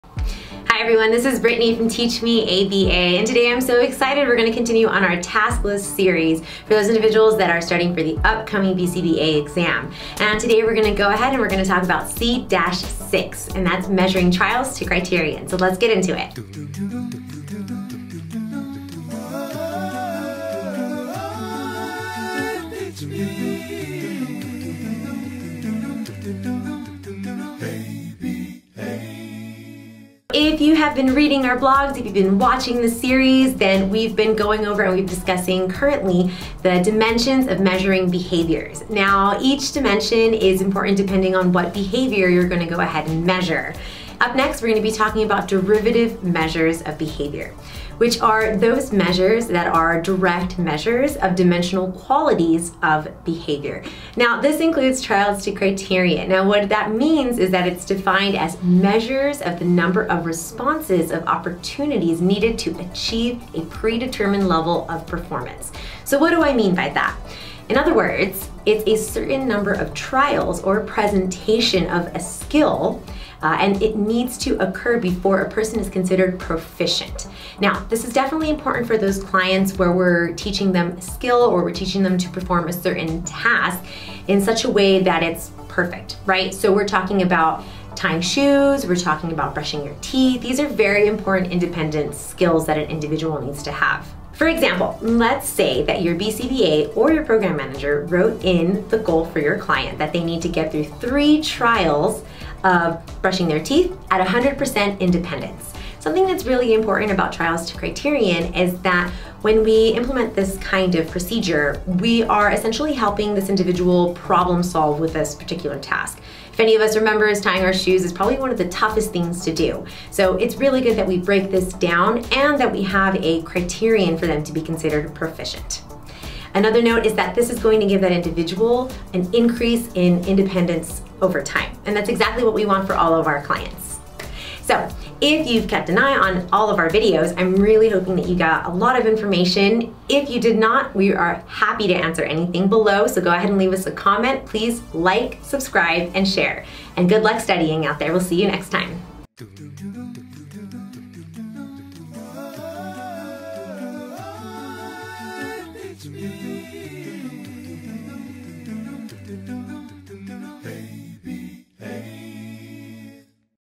BCBA Task List 5: C 6 - Measure trials to criterion.
Derivative measures of behavior are those measures that are direct measures of dimensional quantities of behavior. This includes trials to criterion defined as a measure of the number of response opportunities needed to achieve a predetermined level of performance (Cooper et al., pg. 81, 2020).